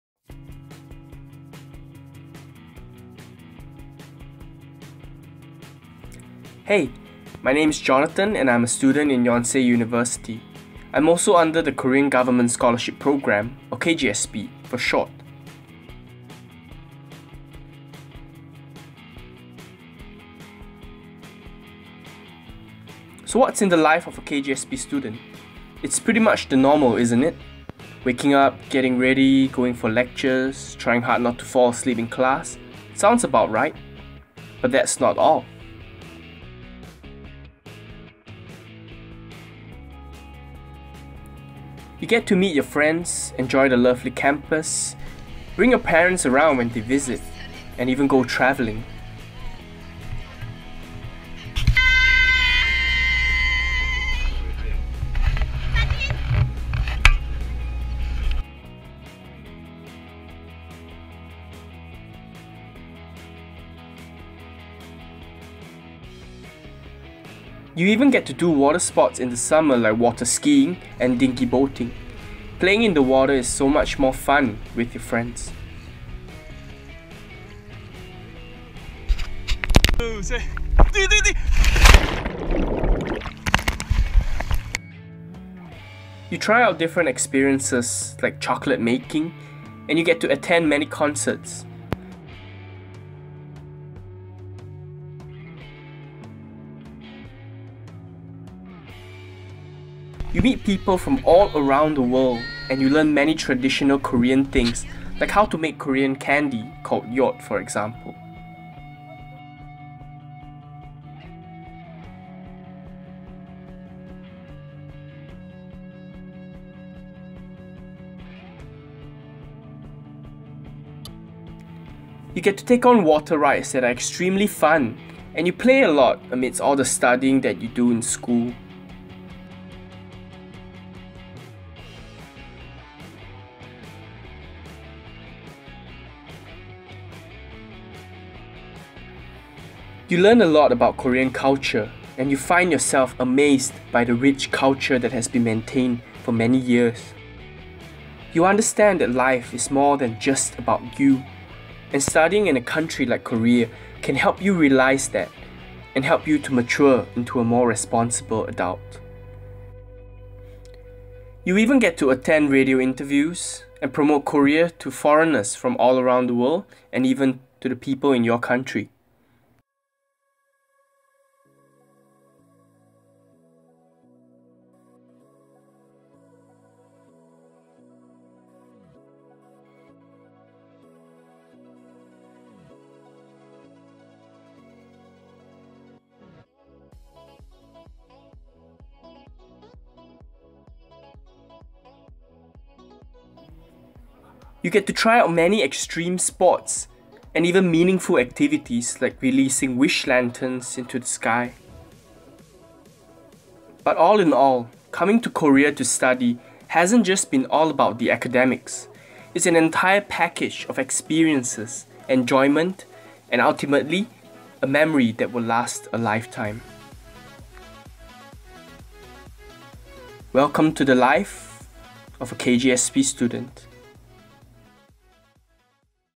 The Life of a KGSP Student 2015
This is a video that I made with regards to being a Korean Government Scholarship Program (KGSP) student in South Korea. Many people think that studying in Korea is all about the books and academics. But here I am to show you, that after 2 years here, it's all about the studies, but it's also all about the experiences.

Entered this into a competition from NIIED and won a participation award (100,000W).